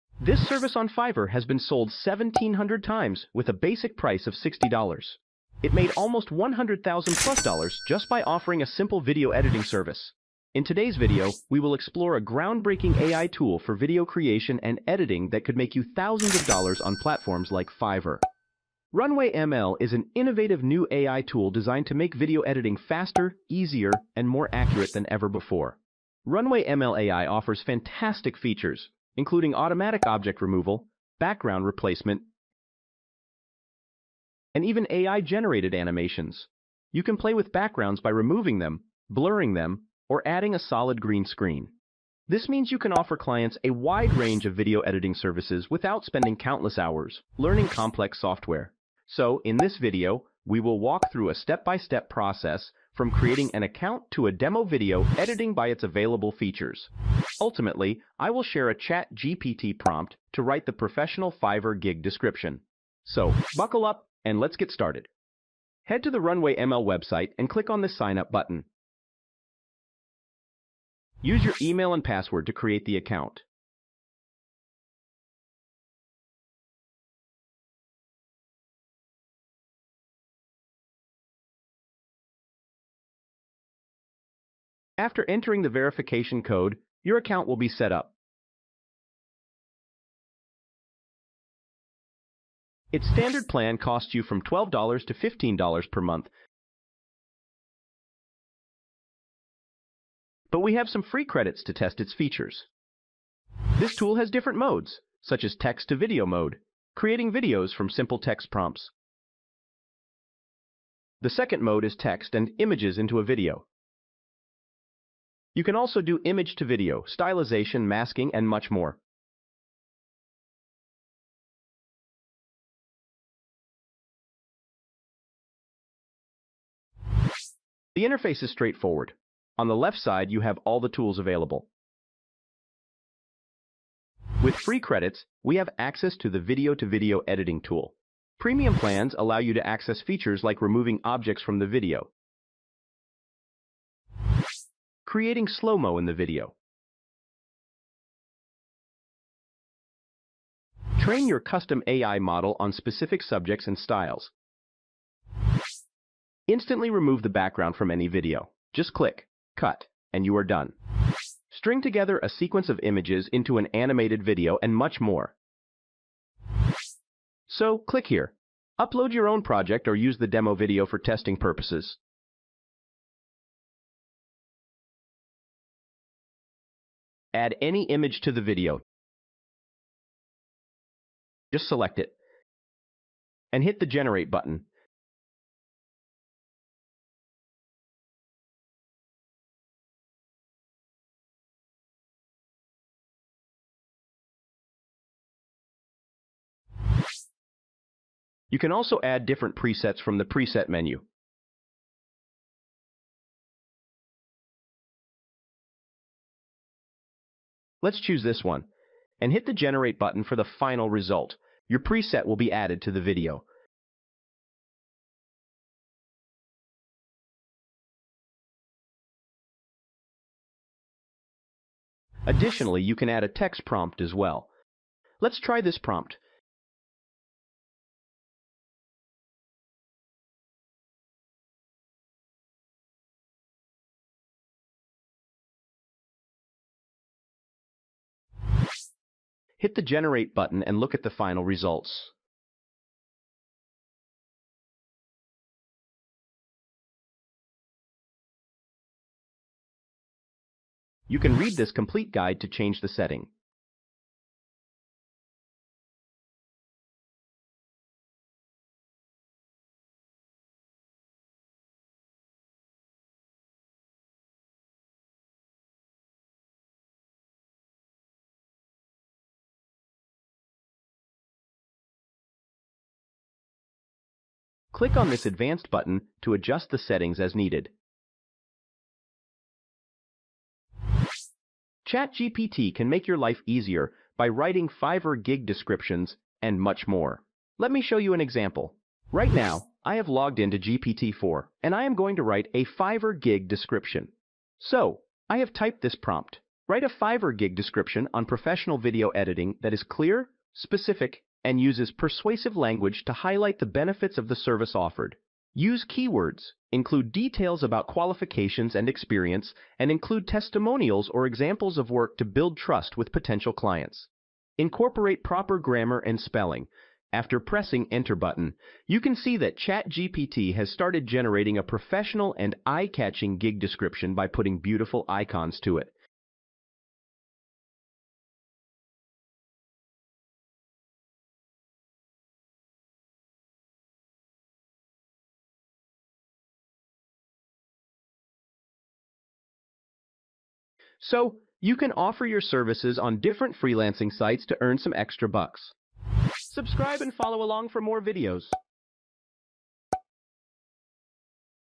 How To Make Passive Income Online with ChatGPT & Runwayml Video Editor
Passive income is the key to financial freedom, and with the rise of technology, it's easier than ever to generate income online.

In this tutorial, you'll learn how to use ChatGPT and Runwayml Video Editor to create content that generates passive income. I'll be walking you through the process step-by-step, from generating ideas to creating and editing your videos.

This channel produce content about ai tools such as chatgpt, midjourney, and other tools.